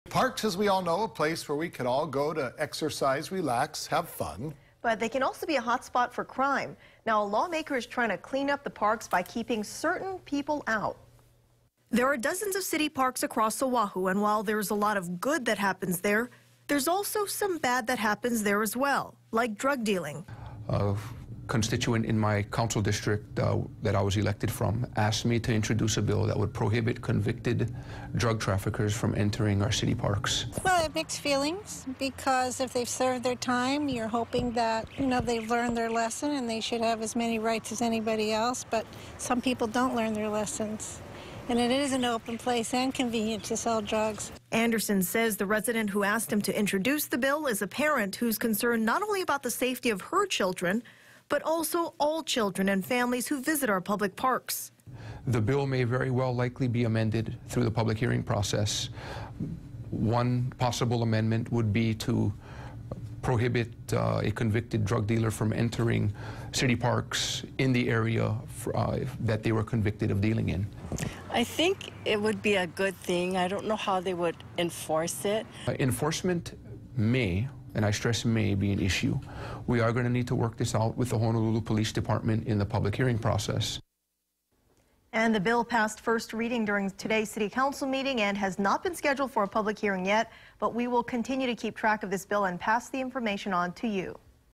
Council bill proposes keeping drug dealers out of parks
Honolulu City Councilman Ikaika Anderson says a parent asked him to introduce the bill.